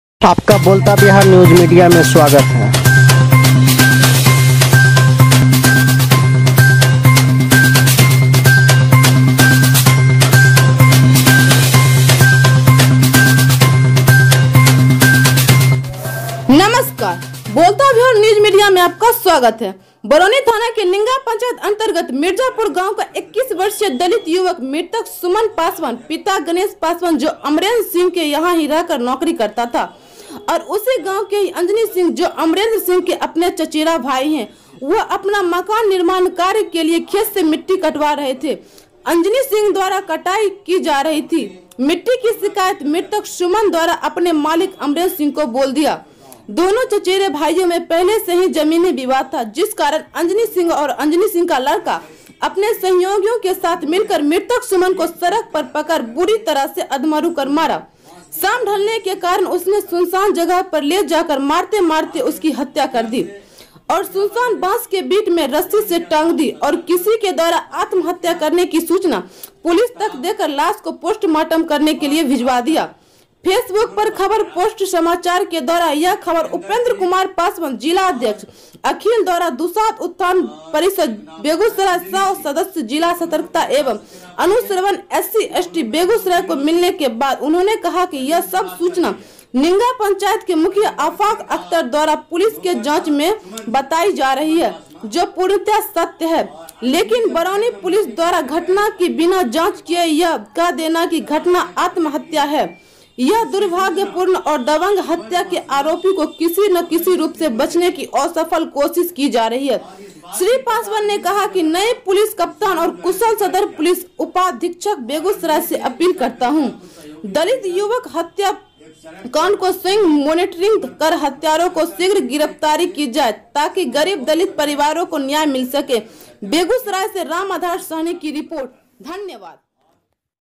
दबंगों ने दलित युवक की हत्या कर शव को बाँसबारी में लटकाया,बरौनी पुलिस केश को मोरना चाहतें हैं।
बेगूसराय से रामाधार सहनी की रिपोर्ट

बरौनी थाना के निंगा पंचायत अंतर्गत मिर्जापुर गांव का 21 वर्षीय दलित युवक मृतक सुमन पासवान पिता गणेश पासवान जो अमरेंद्र सिंह के यहां ही रहकर नोकरी करता था और उसी गांव के ही अंजनी सिंह जो अमरेंद्र सिंह के अपना चाचेरा भाई हैं ,वह अपना मकान निर्माण कार्य के लिये खेत से मिट्टी कटवा रहे थे, अंजनी सिंह द्वारा कटाई जा रही मिट्टी का शिकायत  मृतक सुमन द्वारा अपने मालिक अमरेंद्र सिंह को बोल दिया ,दोनों चचेरे भाइयों में पहले से जमीनी विवाद भी है ,जिस कारण  अंजनी सिंह और अंजनी सिंह का लड़का अपने सहयोगियों के साथ मिलकर मृतक सुमन को सड़क पर पकड़ कर बुरी तरह से अधमरू कर मारा शाम ढ़लने के कारण उसे सुनसान जगह ले जाकर मारते-मारते ,उसकी हत्या कर दी और सुनसान बांस के बीट  में रस्सी से टांग दी,और किसी के द्वारा आत्महत्या करने की सूचना पुलिस देकर लाश को पोस्टमोर्टम के भेजवा दिया ,फेसबुक बुक पर खबर पोस्ट समाचार के द्वारा यह खबर उपेन्द्र कुमार पासवान जिलाध्यक्ष -अखिल भारतीय दुसाध उत्थान परिषद बेगूसराय सह सदस्य जिला सतर्कता एवं अनुश्रवण (sc/st)बेगूसराय  को मिलने के बाद उन्होंने कहा  यह सब सूचना निंगा पंचायत के मुखिया आफाक अख्तर द्वारा पुलिस के जांच में बतायी जा रही है जो पूर्णतः सत्य है ,लेकिन बरौनी पुलिस द्वारा घटना की बिना जांच किये यह कह देना कि -घटना आत्महत्या की  है ,यह दुर्भाग्यपूर्ण और दबंग हत्या के आरोपी को किसी न किसी रूप से बचने की असफल कोशिश की जा रही है ।श्री पासवान ने कहा नये पुलिस कप्तान  और कुशल सदर पुलिस  उपाधीक्षक बेगूसराय से अपील करता हूँ ,दलित युवक हत्या कांड को स्वंय मोनेटरिंग कर हत्यारों को शीघ्र गिरफ्तारी की जाय ताकि गरीब दलित परिवार को न्याय मिल सकें ।